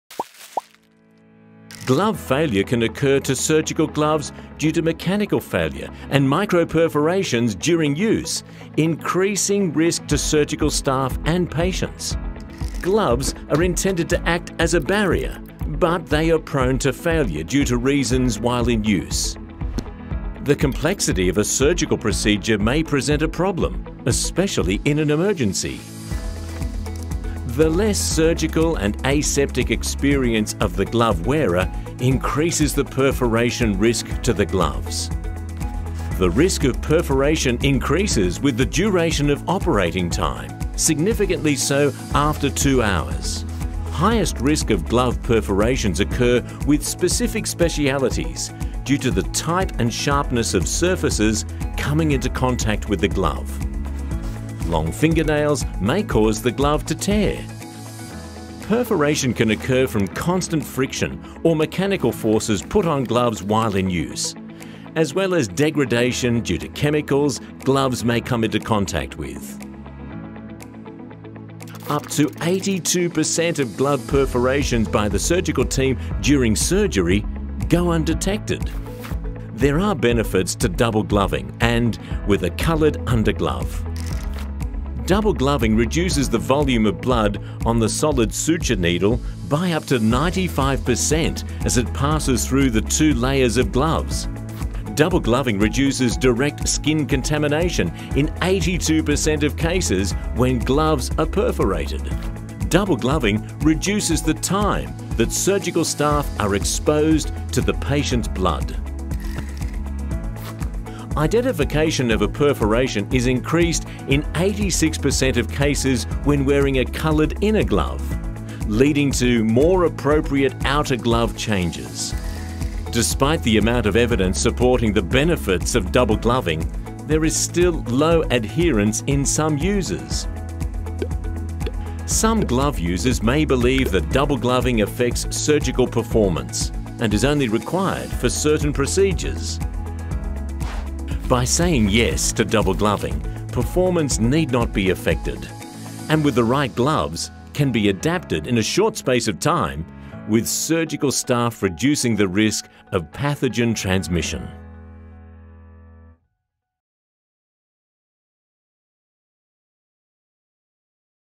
How to Reduce Surgical Glove Failure
Double gloving should be a gold standard and best practice.

Learn about Ansell's pre-donned double gloving system with two gloves in one, making it easy to double glove in one don